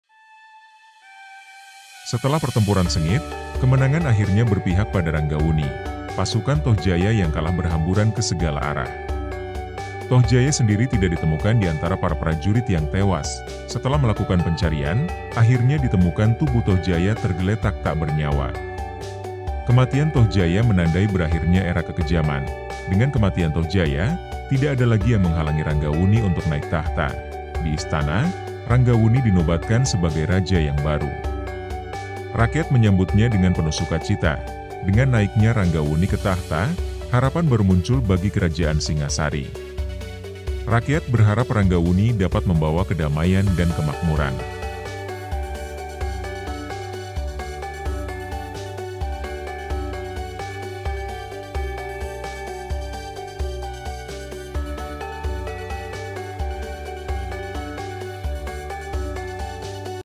Ranggawuni 16
Ranggawuni dinobatkan sebagai raja yang baru. Rakyat menyambutnya dengan penuh suka cita.

Legenda rasa, warisan budaya @keripik.legenda

Follow @amazingmalang, media informasi terbesar di Malang Raya.

Digital Agency terbaik di Malang, @optimaise.id ✅